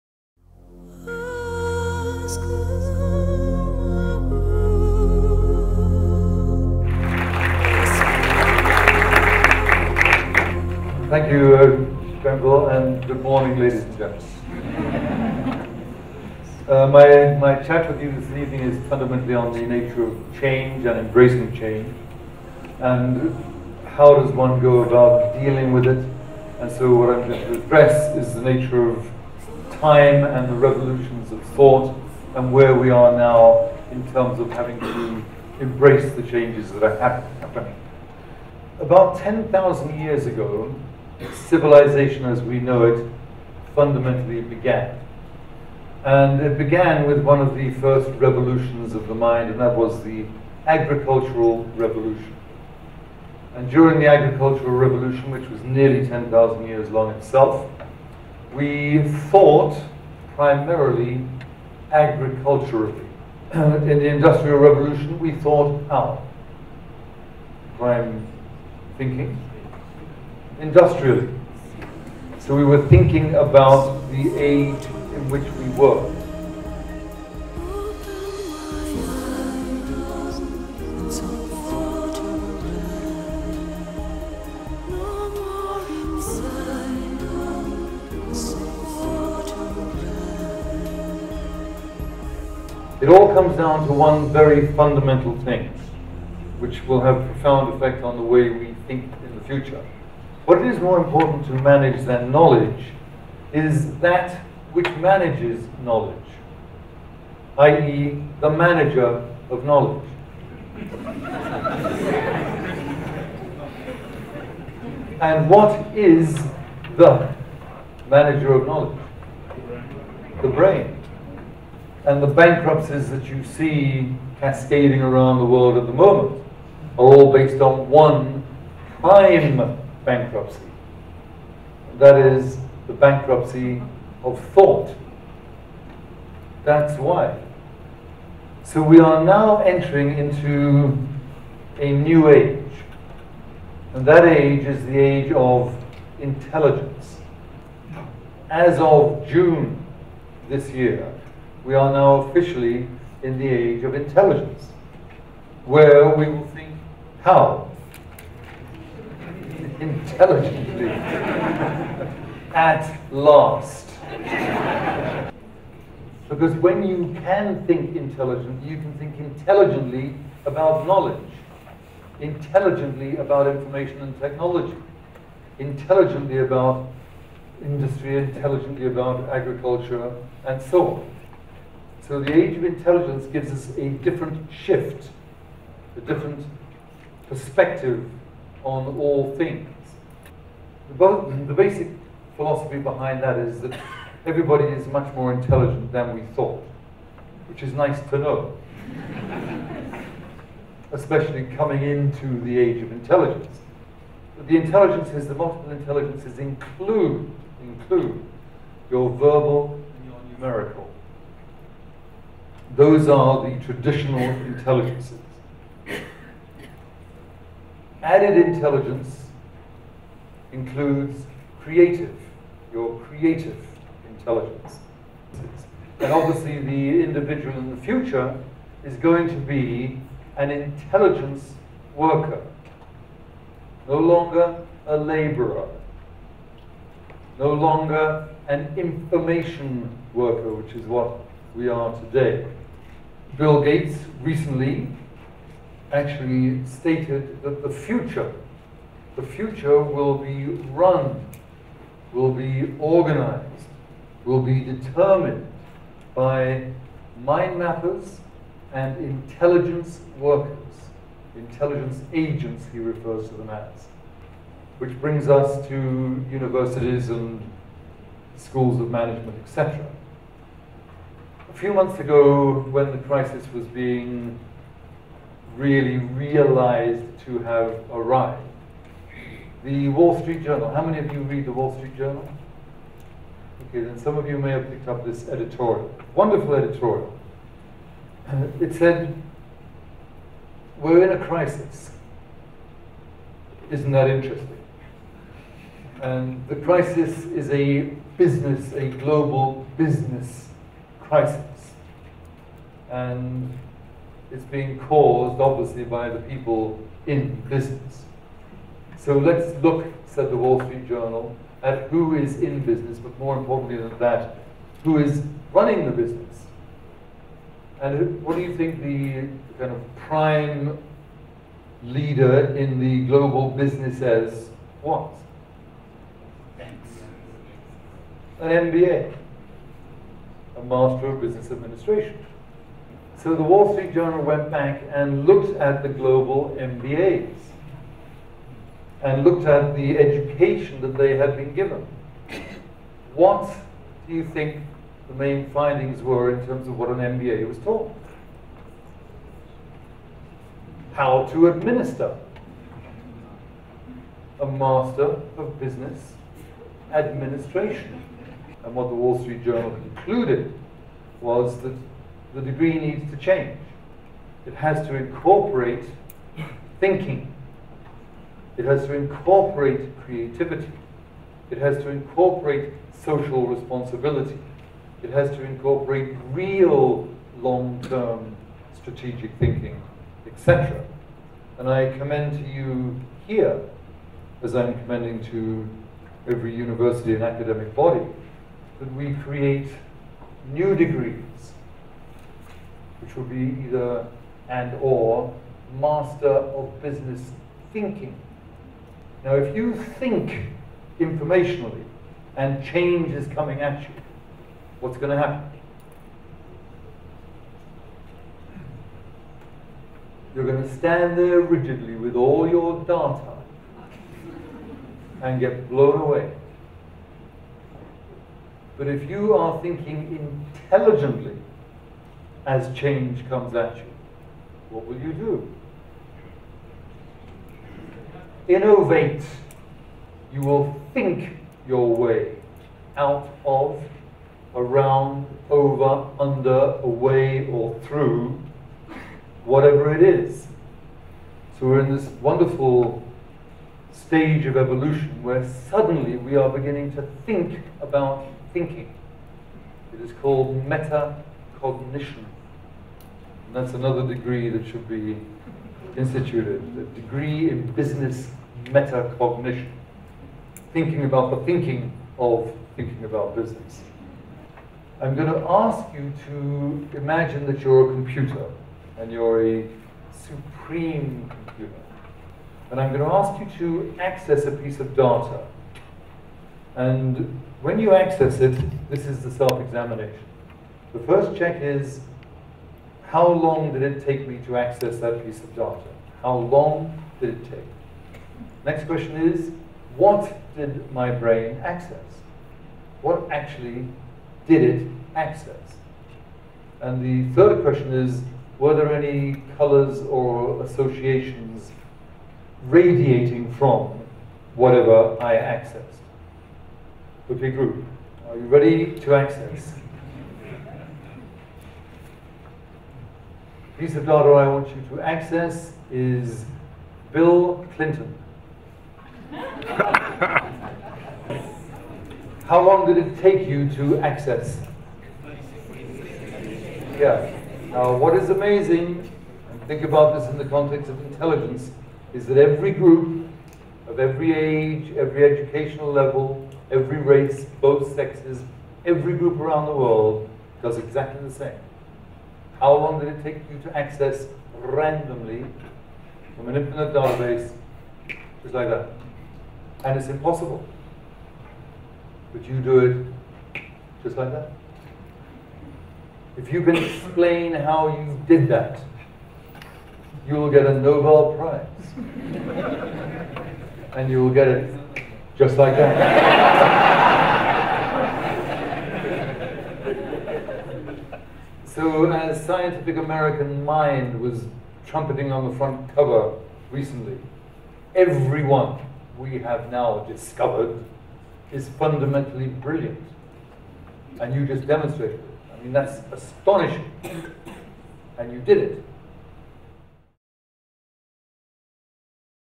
WBS Tony Buzan "The Intelligence Age"
Embracing Change - It begins with You.�
Hosted by Wits Business School
Date: Tuesday, 4 August 2009
Time: 17h00 for 17h30
Tony Buzan, creator of Mind Maps, believes every individual has the potential for brilliance.

Our thought processes have been shaped by the different Ages mankind has experienced. In the Agricultural Age we thought agriculturally, in the Industrial Age we thought industrially. Industrial thinking is "accepting, no thought allowed and clone-like".
The recently ended Information/Technology Age was characterised by a focus on technological delivery and collection of information

Buzan says that the advent of the "information overload" was the death-knell of the Information/Technology Age. Welcome to the Intelligence Age.